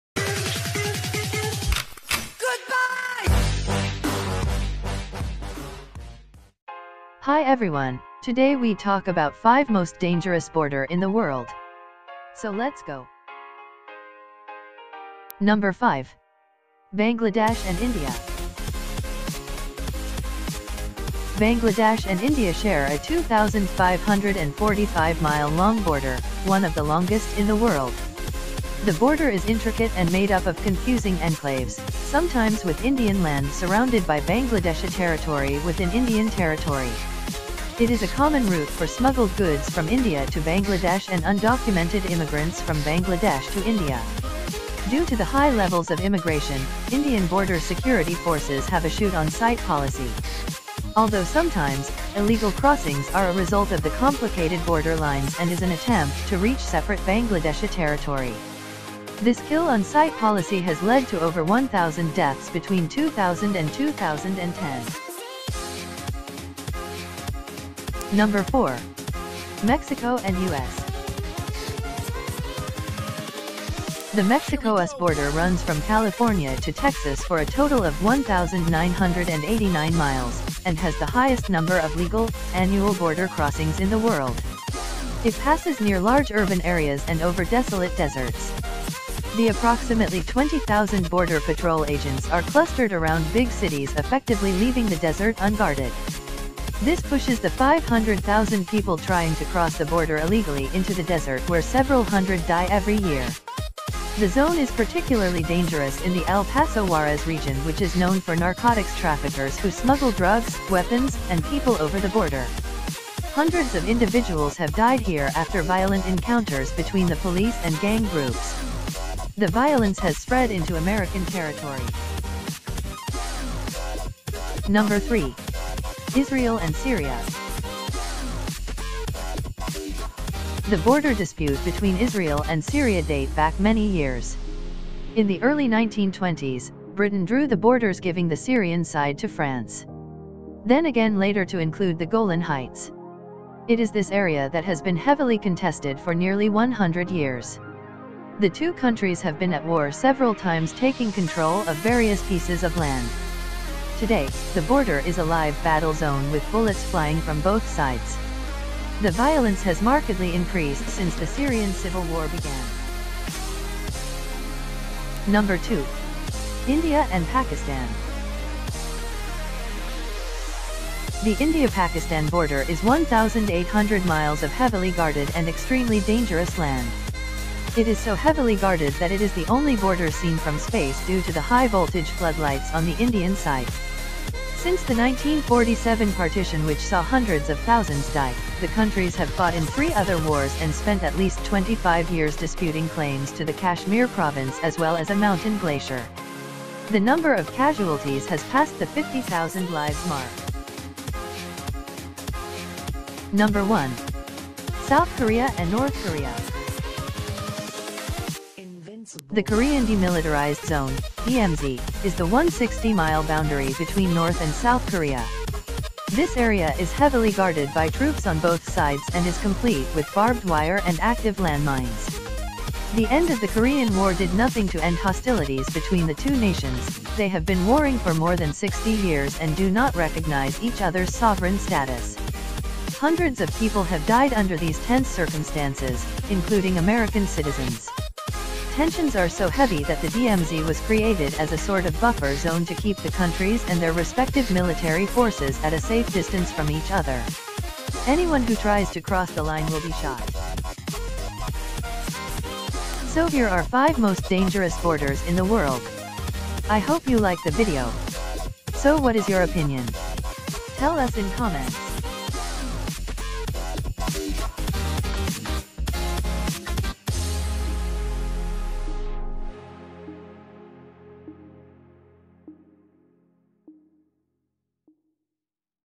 5 Most Dangerous Border in the World
Hi Everyone, Today Wee Talk About 5 Most Dangerous Border in the World. So let's go.

Number 5. Bangladesh/India

Bangladesh and India share a 2,545 mile long border, one of the longest in the world. The border is intricate and made up of confusing enclaves, sometimes with Indian land surrounded by Bangladeshi territory within Indian territory! It is a common route for smuggled goods from India to Bangladesh and undocumented immigrants from Bangladesh to India. Due to the high levels of immigration, Indian Border Security Forces have a shoot on sight policy. Although sometimes, “illegal crossings” are a result of the complicated border lines and is an attempt to reach separate Bangladeshi territory. This kill on sight policy has led to over 1,000 deaths between 2000 and 2010.

Number 4. Mexico/US

The Mexico-US border runs from California to Texas for a total of 1,989 miles, and has the highest number of legal, annual border crossings in the world. It passes near large urban areas and over desolate deserts. The approximately 20,000 border patrol agents are clustered around big cities effectively leaving the desert unguarded. This pushes the 500,000 people trying to cross the border illegally into the desert where several hundred die every year. The zone is particularly dangerous in the El Paso-Juarez region which is known for narcotics traffickers who smuggle drugs, weapons, and people over the border. Hundreds of individuals have died here after violent encounters between the police and gang groups. The violence has spread into American territory.

Number 3. Israel/Syria

The border dispute between Israel and Syria date back many years. In the early 1920s, Britain drew the borders giving the Syrian side to France. Then again later to include the Golan Heights. It is this area that has been heavily contested for nearly 100 years. The two countries have been at war several times taking control of various pieces of land. Today, the border is a live battle zone with bullets flying from both sides. The violence has markedly increased since the Syrian Civil War began.

Number 2. India/Pakistan

The India-Pakistan border is 1,800 miles of heavily guarded and extremely dangerous land. It is so heavily guarded that it is the only border seen from space due to the high voltage flood lights on the Indian side. Since the 1947 partition which saw hundreds of thousands die, the countries have fought in 3 other wars and spent at least 25 years disputing claims to the Kashmir province as well as a mountain glacier. The number of casualties has passed the 50,000 lives mark.

Number 1. South Korea/North Korea

The Korean Demilitarized Zone (DMZ) is the 160-mile boundary between North and South Korea. This area is heavily guarded by troops on both sides and is complete with barbed wire and active landmines. The end of the Korean War did nothing to end hostilities between the two nations, they have been warring for more than 60 years and do not recognize each other’s sovereign status. Hundreds of people have died under these tense circumstances, including American citizens. Tensions are so heavy that the DMZ was created as a sort of buffer zone to keep the countries and their respective military forces at a safe distance from each other. Anyone who tries to cross the line will be shot.

So here are 5 Most Dangerous Borders In The World. I hope you like the video. So What is your opinion. Tell us in comments.